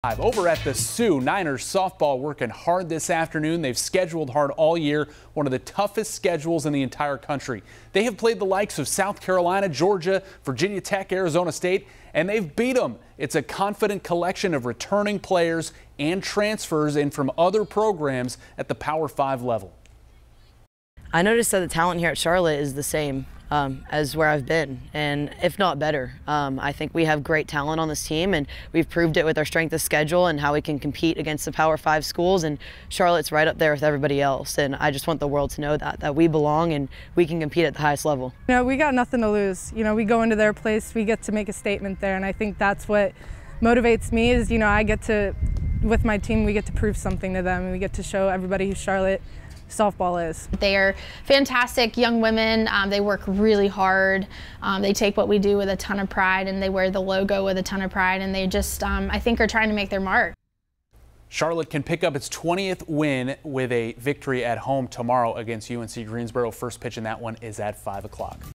Charlotte softball scheduling tough
The Charlotte 49ers softball team recently came in at No. 1 in terms of toughest non-conference schedules and are No. 25 in RPI.

It's all part of the team's goal to make the NCAA postseason field out of the C-USA, whether they win the league tournament or not.

But of course, they're good enough to do that anyway.